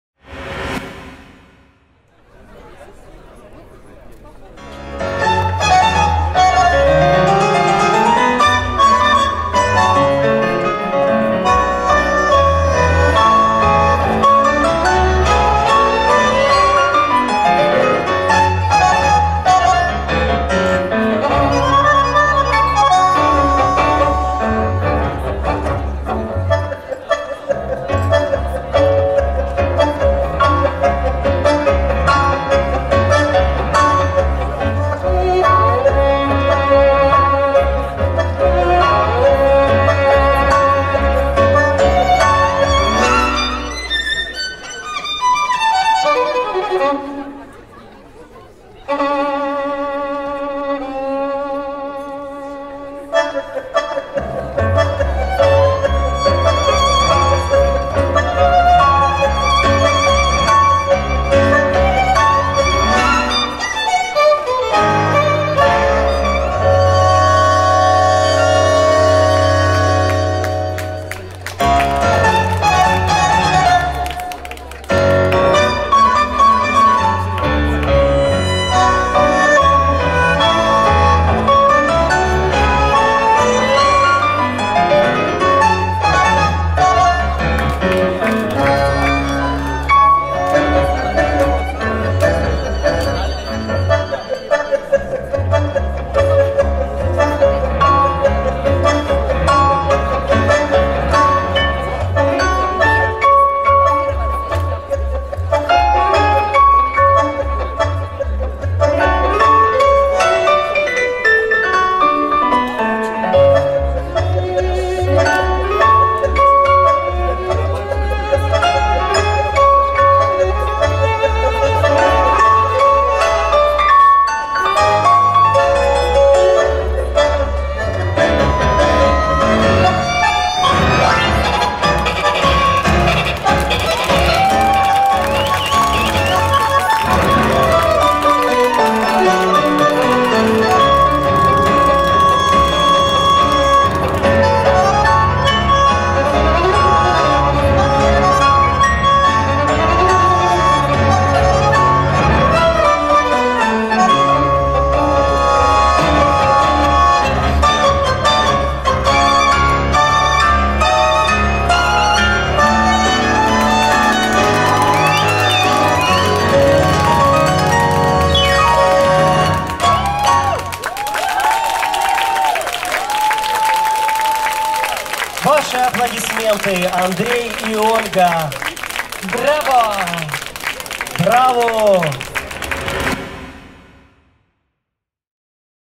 BIG Pro-Am Dance Open Air 2020.Show .Миринда Андрей – Ошкало Ольга
Выступления  в 3-м отделении танцевального мероприятия-BIG Pro-Am Dance Open Air 2020

Show .Миринда Андрей – Ошкало Ольга